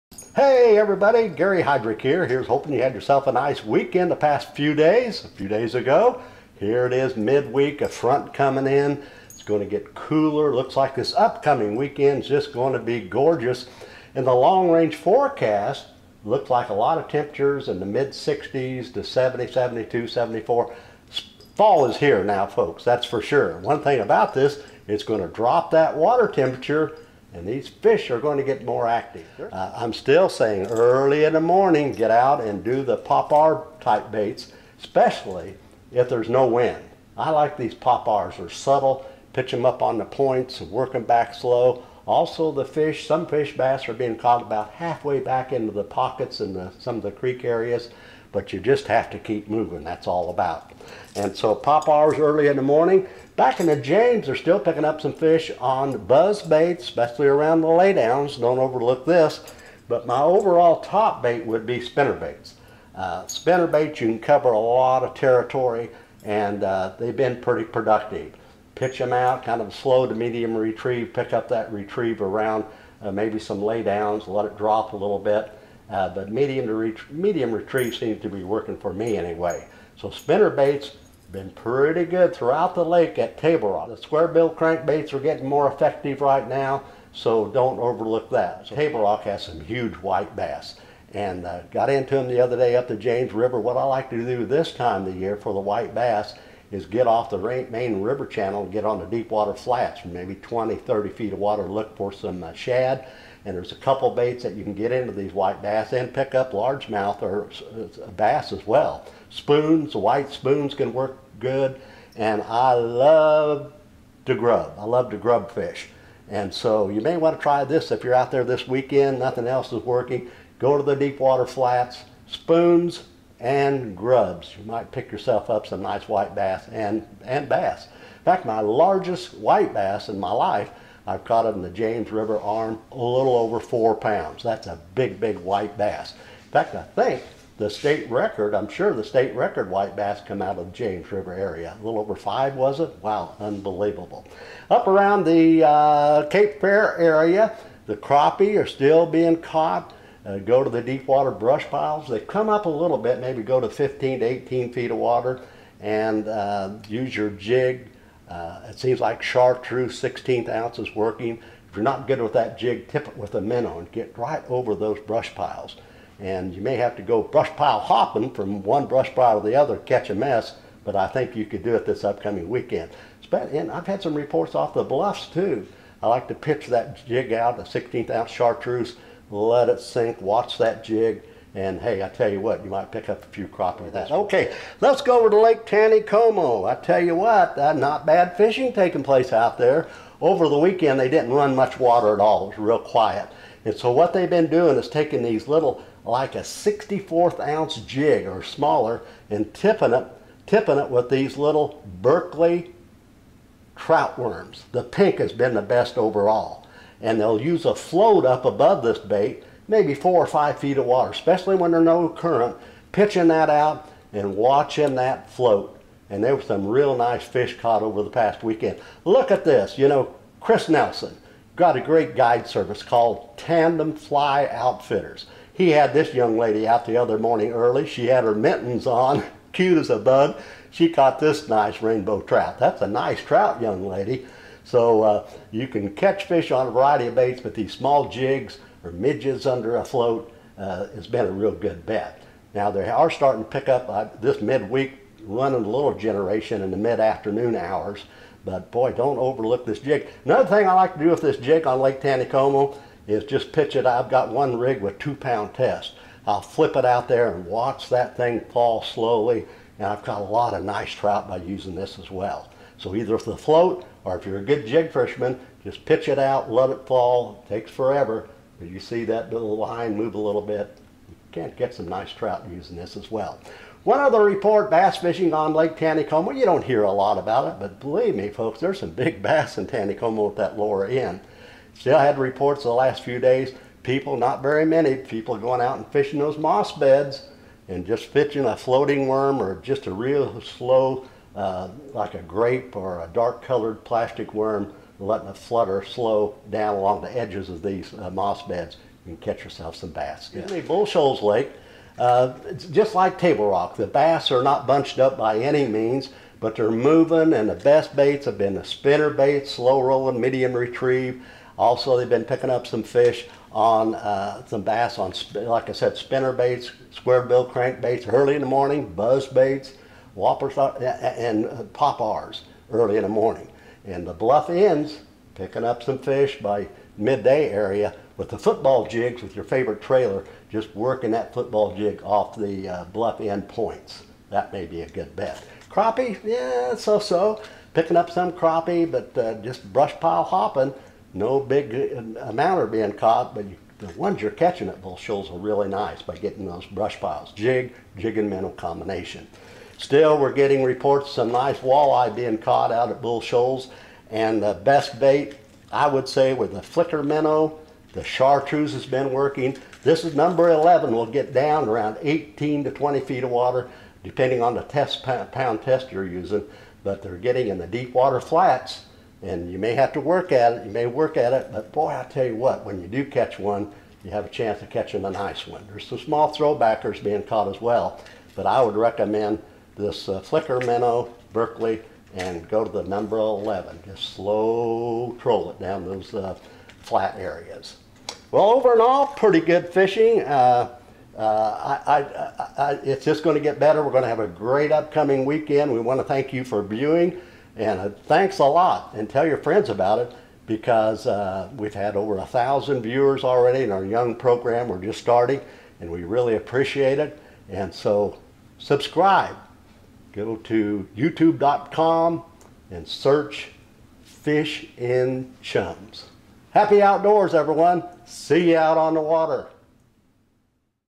Weekly Fishing Report - October 20th, 2016
Watch this report which covers Table Rock Lake, Lake Taneycomo, and Bull Shoals Lake and find out what the Bass, Walleye, and Crappie are going after. We are located in Branson Missouri and we hope that you will tune in to watch these videos.